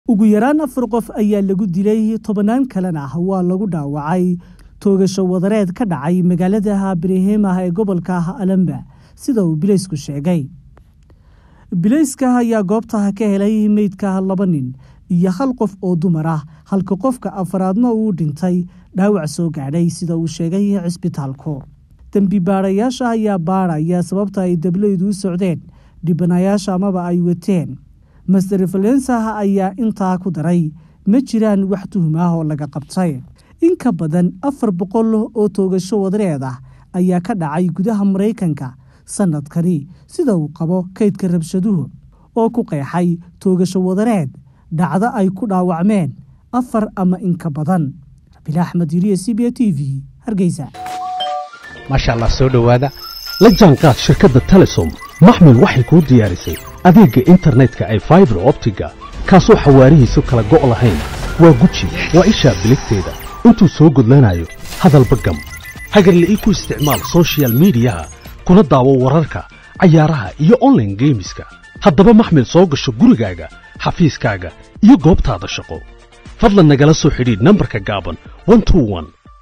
Ugu yaraan afar qof ayaa lagu dilay tobanaan kalena waa lagu dhaawacay toogasho wadareed ka dhacday
Ugu yaraan afar qof ayaa lagu dilay tobanaan kalena waa lagu dhaawacay toogasho wadareed ka dhacday magaalada Birmingham ee gobolka Alabama, USA.